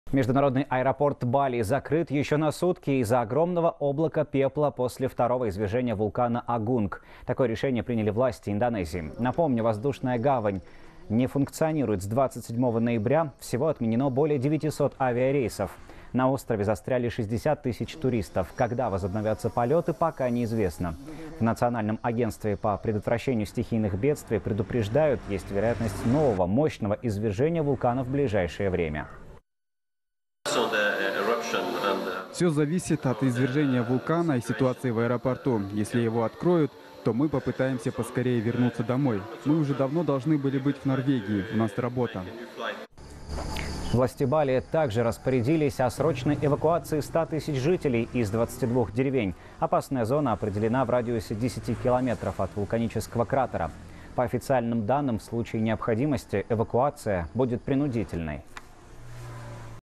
Аэропорт Бали закрыли еще на сутки
Международный аэропорт Бали закрыт еще на сутки из-за огромного облака пепла после второго извержения вулкана Агунг.